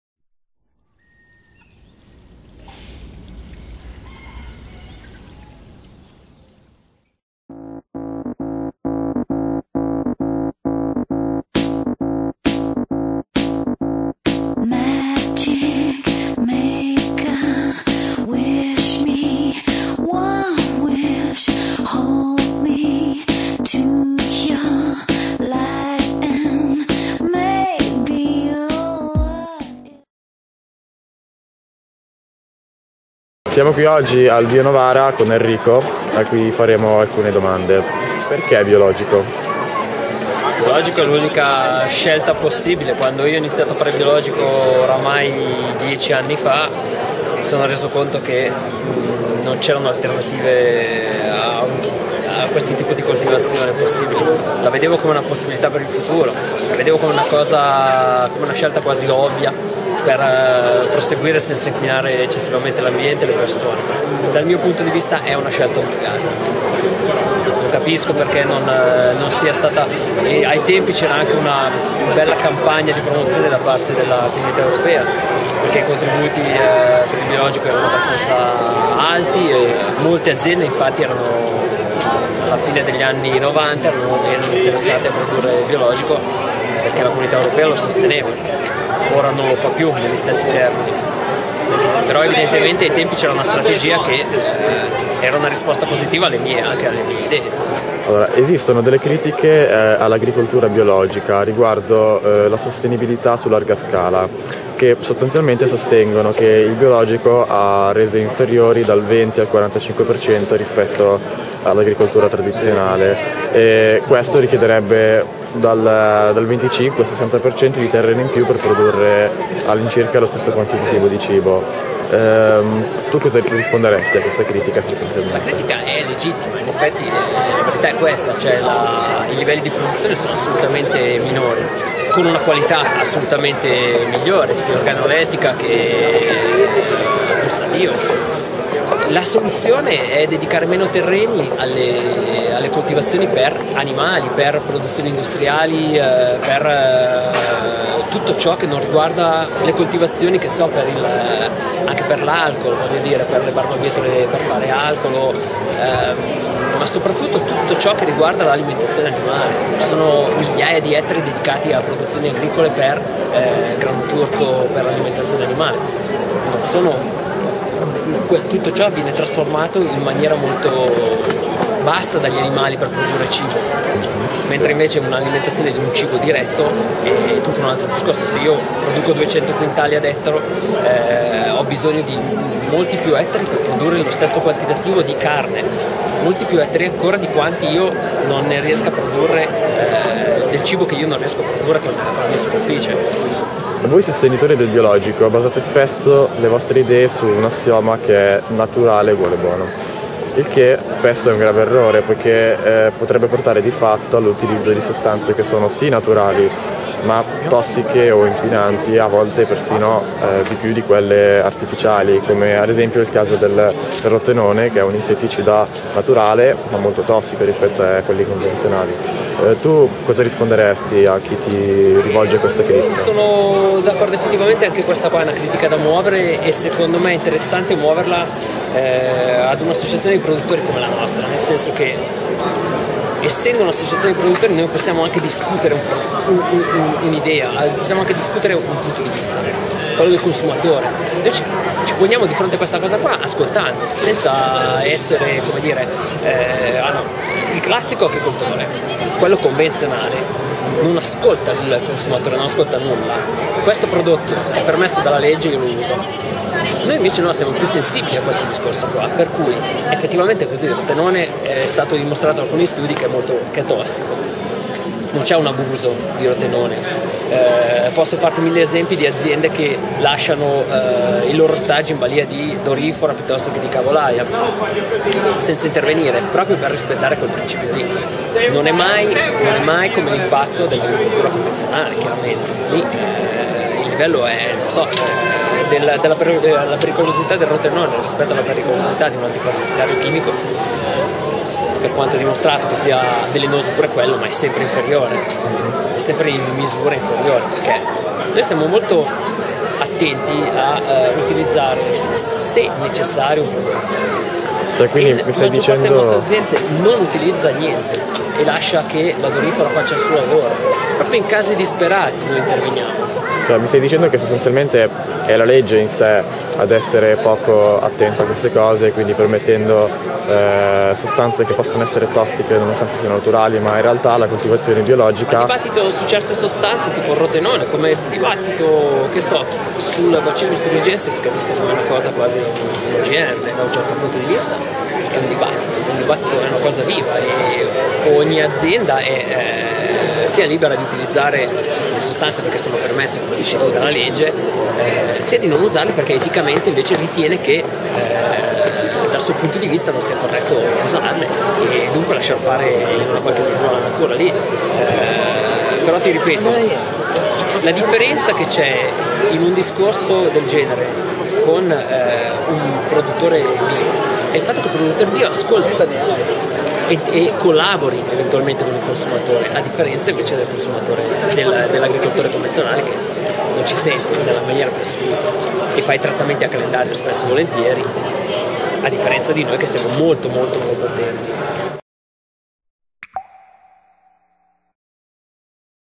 Bionovara in festa: Il Biologico, Intervista ad Enrico Imbevuti.
Durante l'edizione 2010 del "Bionovara in Festa", abbiamo incontrato Enrico Imbevuti, il presidente dell'associazione Bionovara, che ci ha spiegato che cos'è il biologico, e ha risposto alle maggiori critiche che vengono di consuetudine rivolte a questo tipo d'agricoltura, e stile di vita.